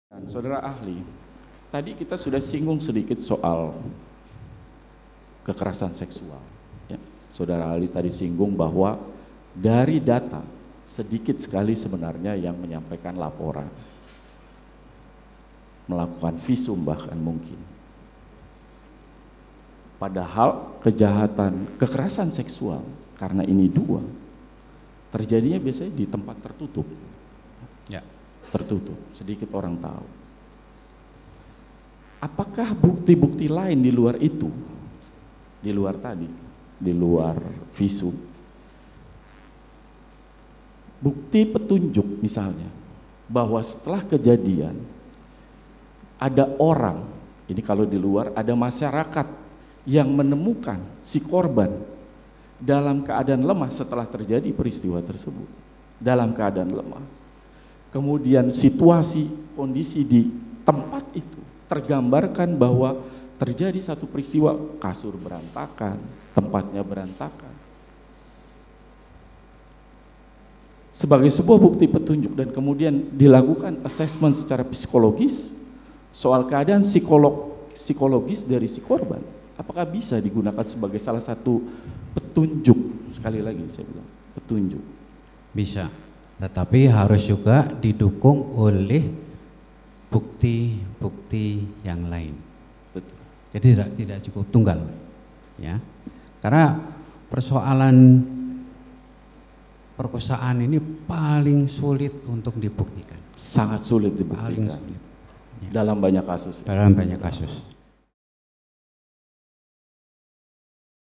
Pengacara Putri Candrawathi Ada Bukti Lain Kekerasan Seksual di Magelang
JAKARTA, KOMPAS.TV - Pengacara Putri Candrawathi, Rasamala Aritonang mengklaim bahwa ada bukti lain yang menguatkan adanya kejadian kekerasan seksual terhadap kliennya tersebut.

Rasamala menyebut bukti itu berasal dari dari hasil psikologi forensik dan bukti keterangan saksi lain.

Ada pula bukti lain yang barasal dari hasil asesmen psikologi forensik yang berada di bawah Asosiasi Psikolgi Forensik Indonesia (Apsifor) yang menunjukkan adanya indikasi kuat terjadi pelecehan seksual. (*)